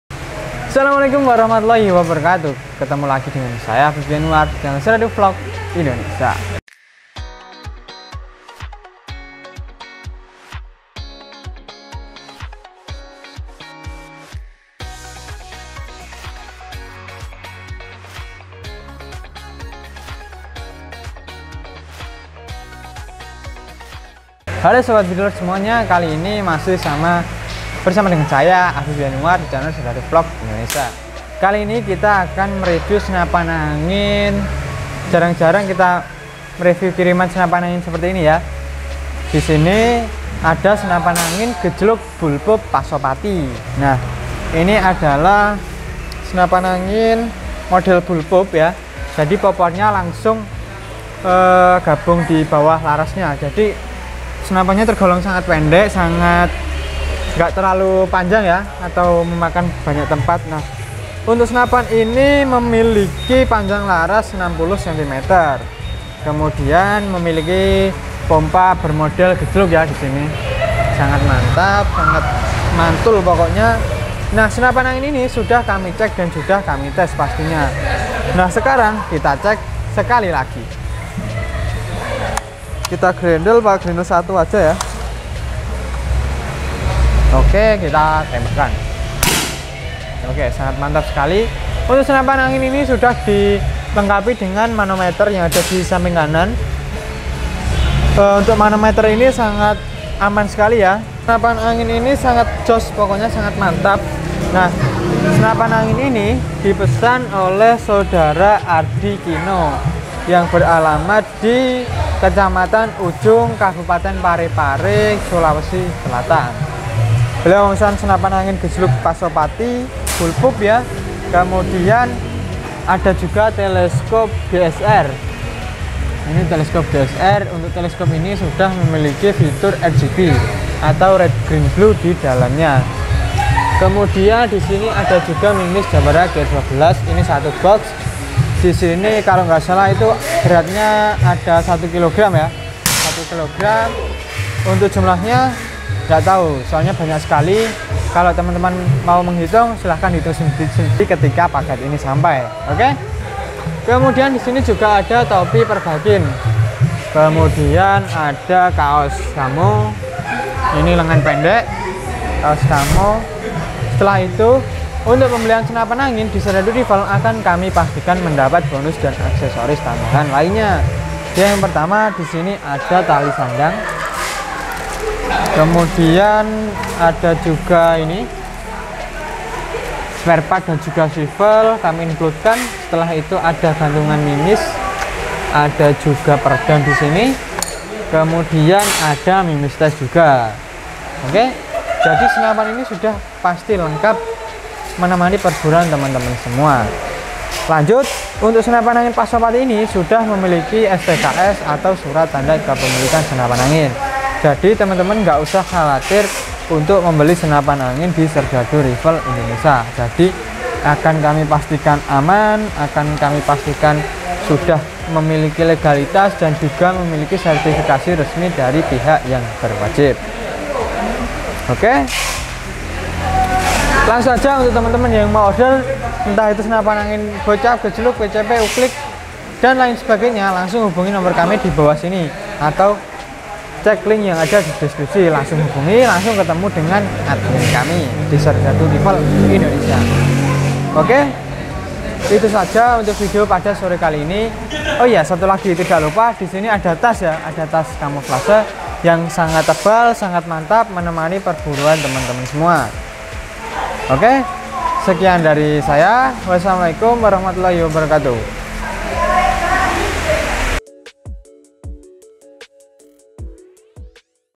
GAHAR!!! Gejluk Bullpup PASOPATI || [Siap kirim ke Parepare]
Halo Para Serdadu Bedhil Indonesia !
Kali ini saya akan mereview kiriman kepada Bapak Ardi di Kota Parepare yang memesan senapan angin
oke Check This Out!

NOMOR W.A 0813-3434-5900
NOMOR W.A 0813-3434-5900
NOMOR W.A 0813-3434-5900